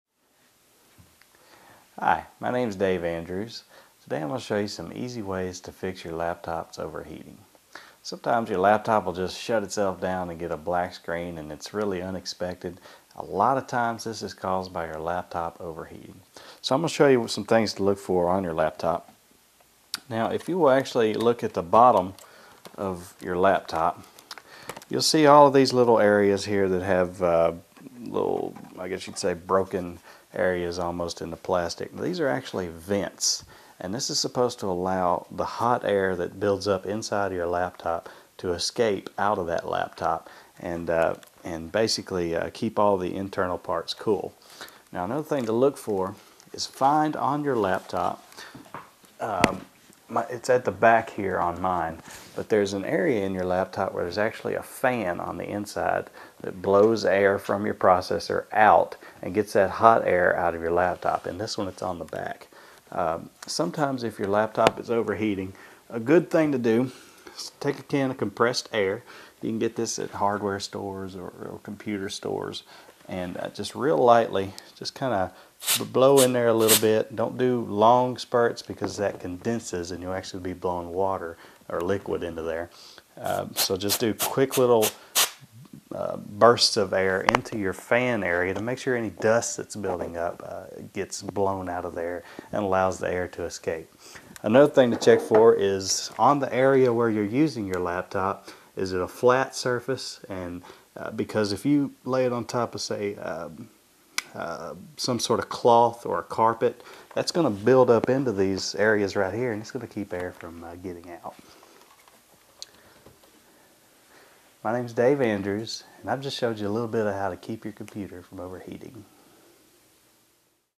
Computers & Laptops : Easy Ways to Fix Laptop Overheating
An easy way to fix laptop overheating is to make sure it rests on a flat surface, make sure the internal fan is working properly and even spray a bit of compressed air into the vent to remove accumulated dust. Keep a laptop from overheating with IT help from a software developer in this free video on computers.